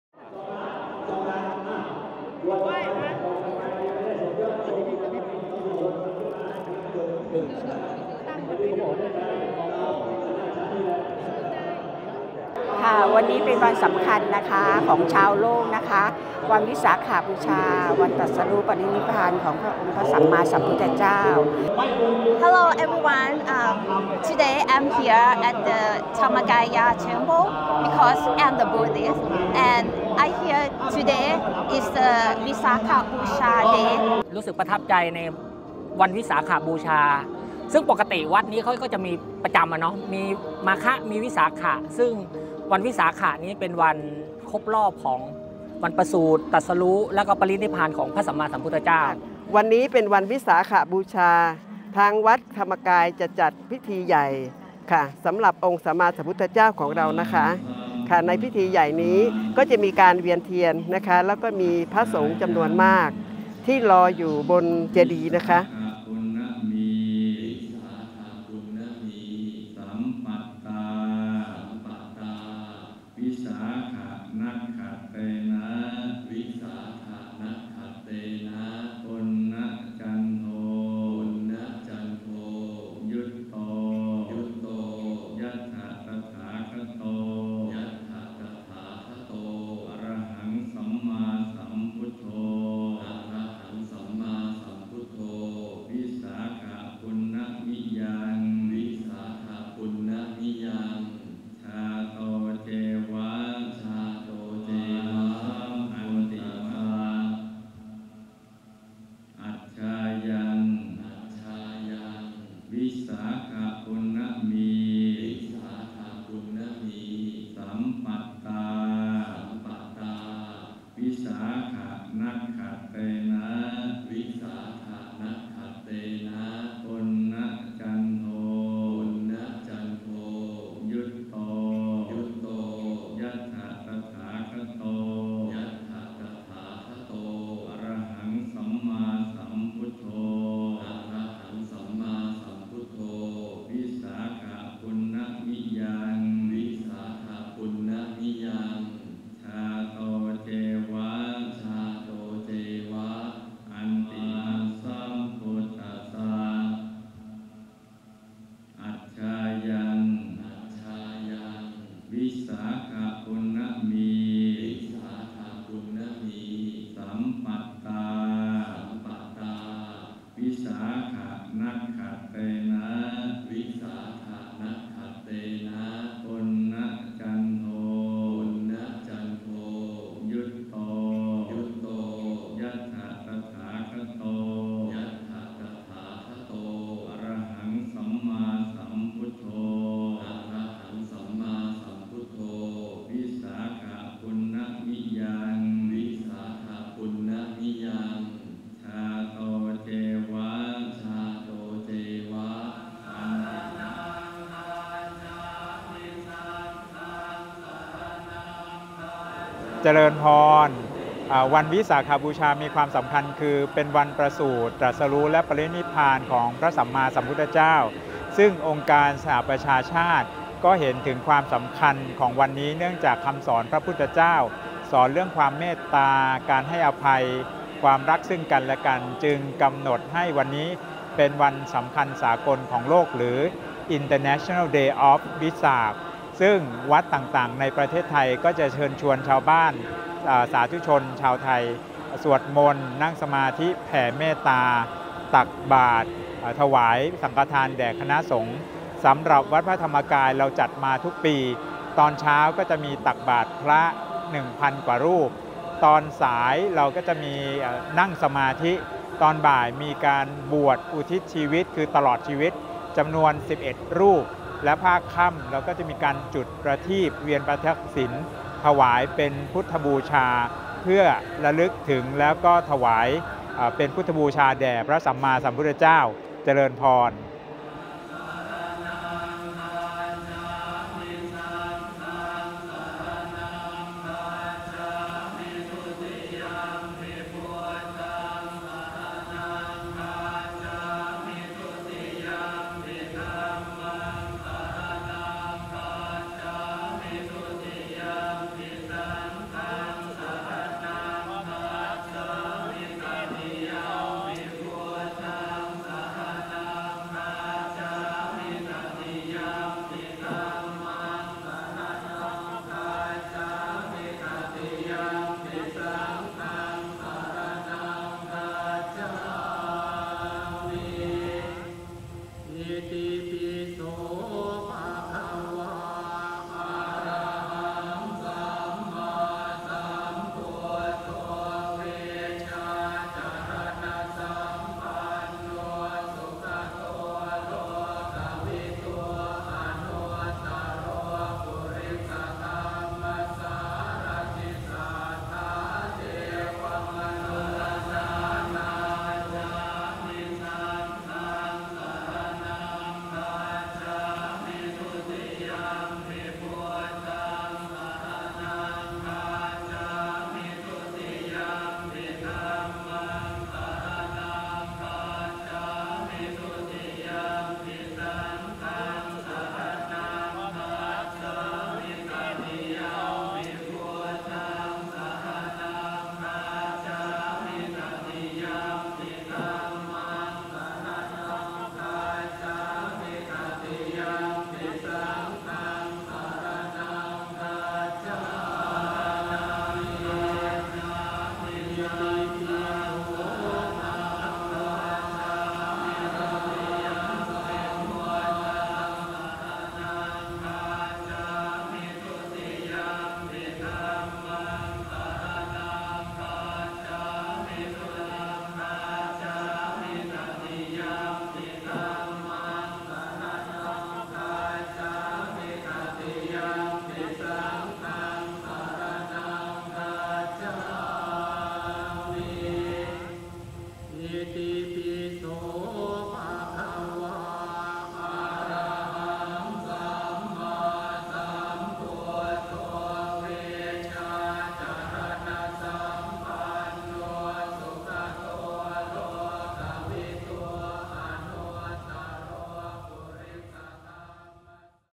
Visakha Bucha Day May 22 , 2024 at Wat Pra Dhammakaya Temple - A Film by David Found
Drone footage supplied Dhammakaya Temple.